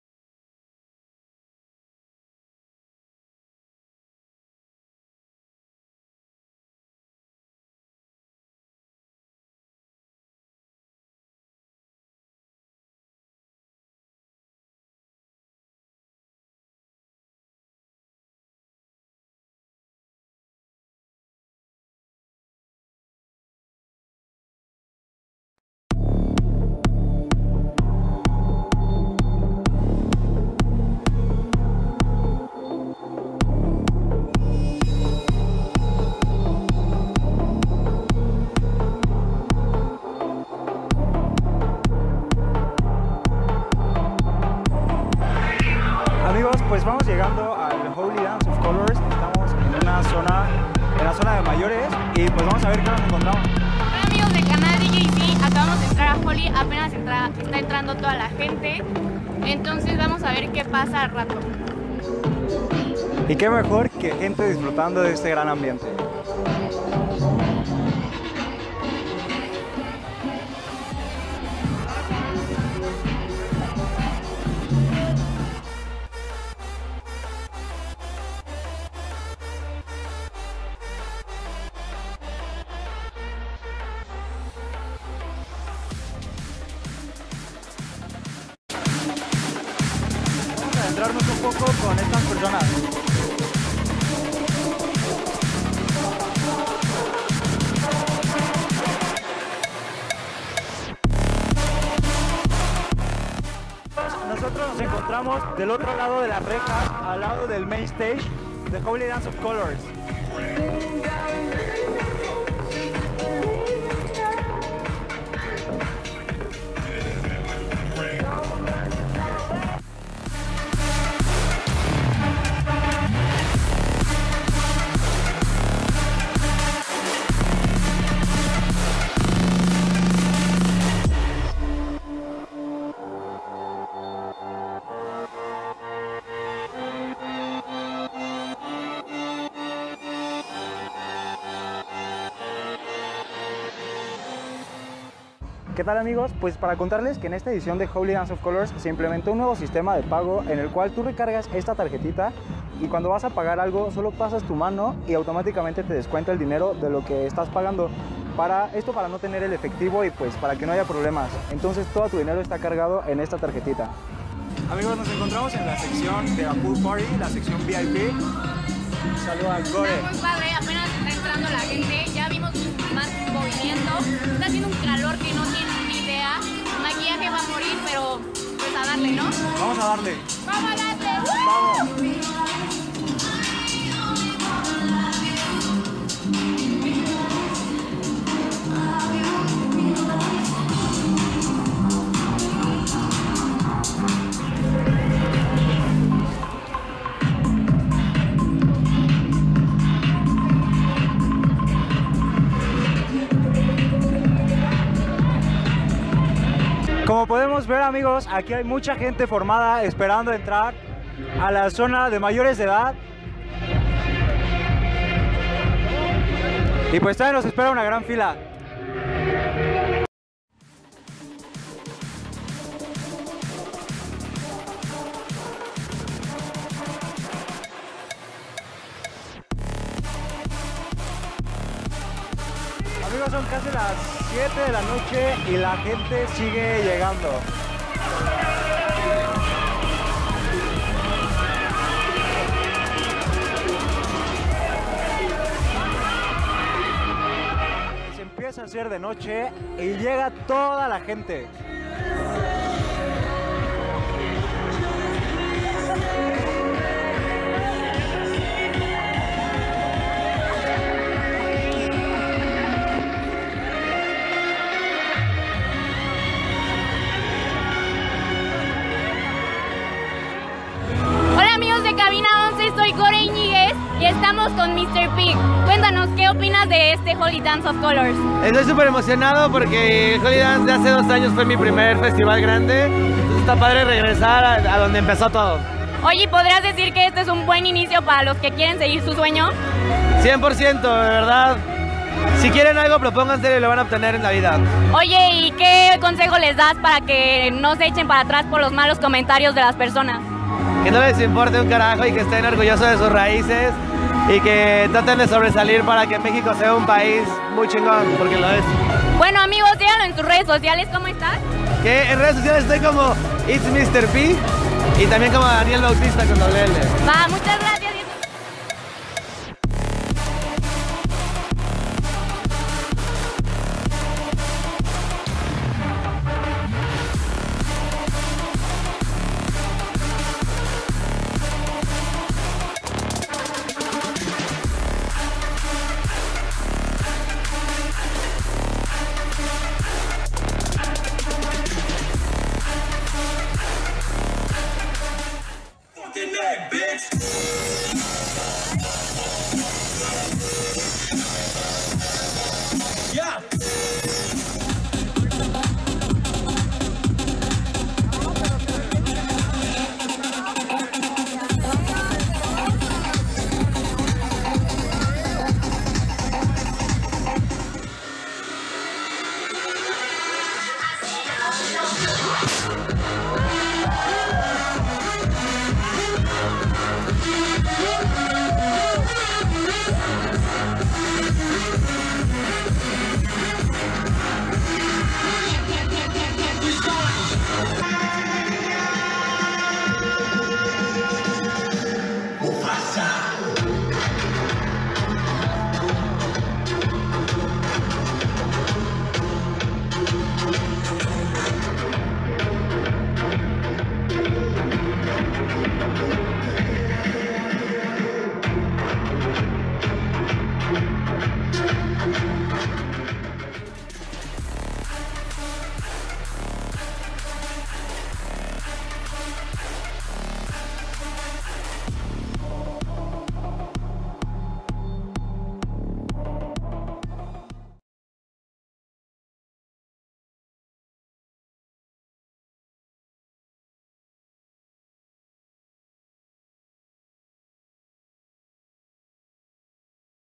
"Yei sii conoce" en esta ocasión se fue a Holli Dance, solo por Cabina 11.
En este programa, recordaremos los que ocurrió en el mejor festival de música electrónica, así que, ¡acompáñanos!  Solo por, Cabina 11.